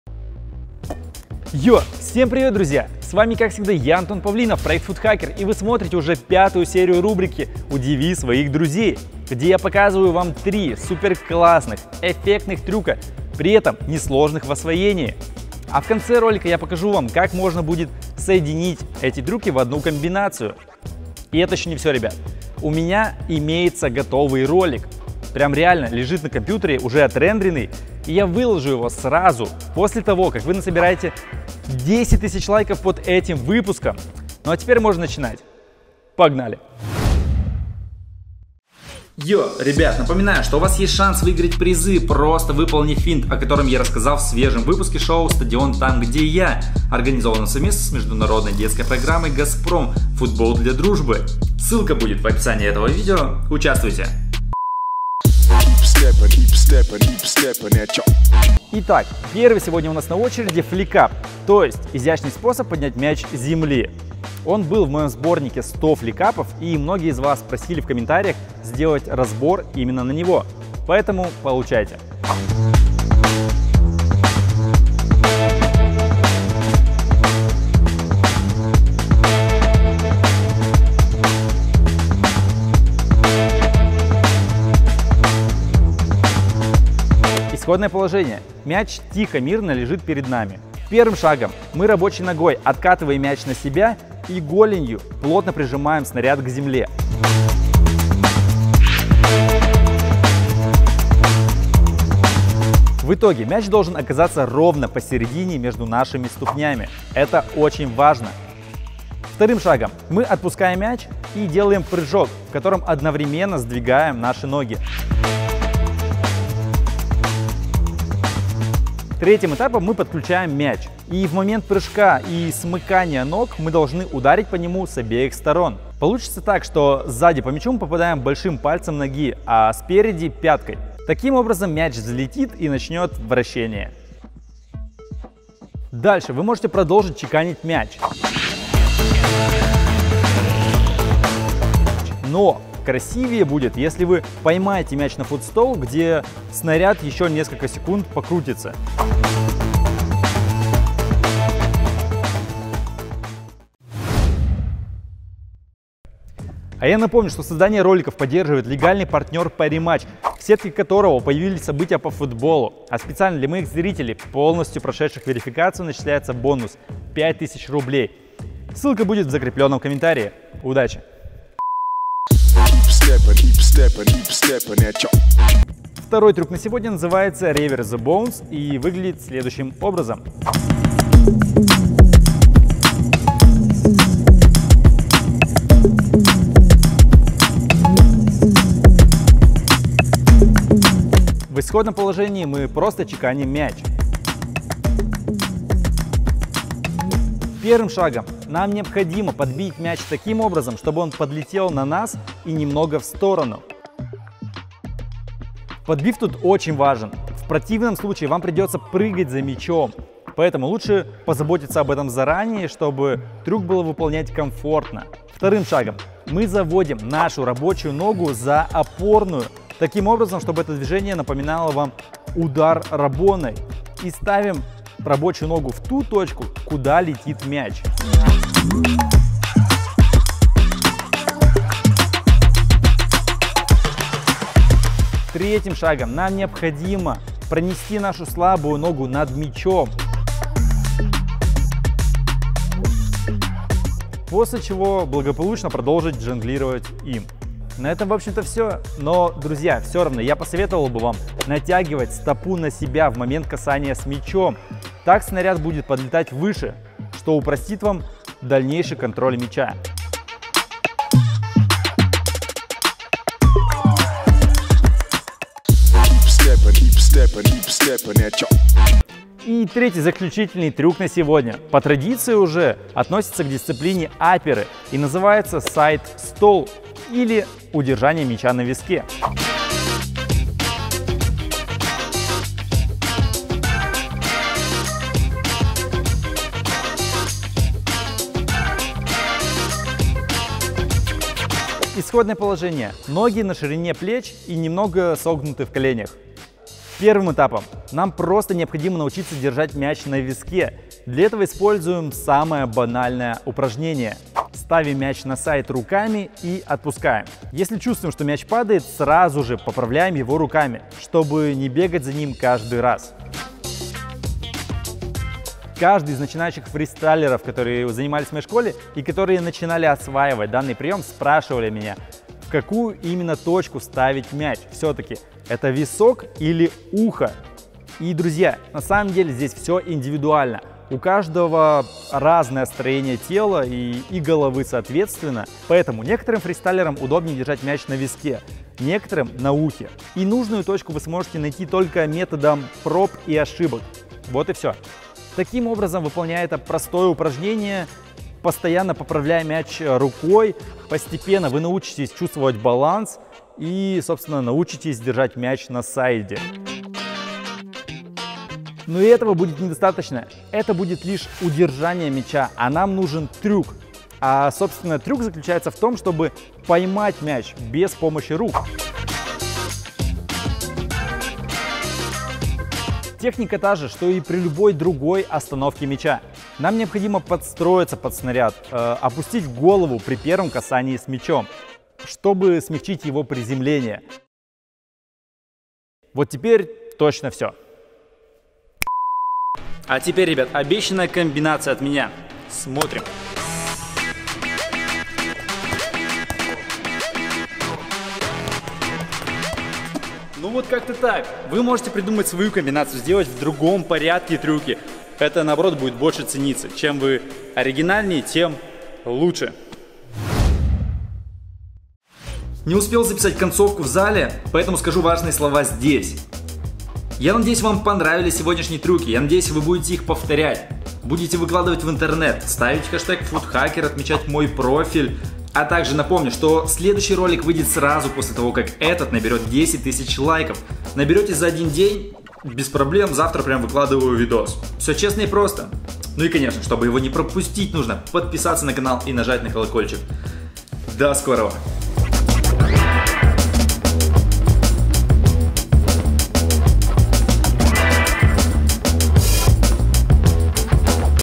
УДИВИ СВОИХ ДРУЗЕЙ #5 / Три простых трюка обучение / Фристайл Футбол
Всем привет, друзья! С вами как всегда я Антон Павлинов, проект футхакер и вы смотрите пятую серию рубрики "удиви своих друзей"! Где я показываю вам три супер классных эффектных трюка, при этом не сложных в освоении.